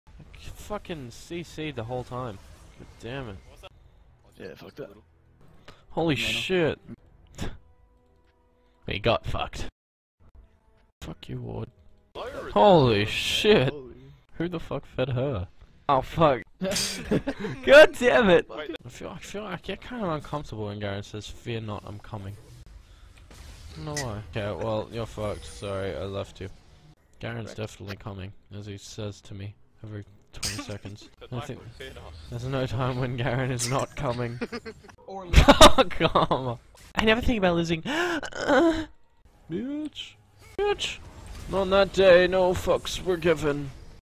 Michael Clifford Swearing
Video compiled of different parts from the second twitch livestream. Enjoy :)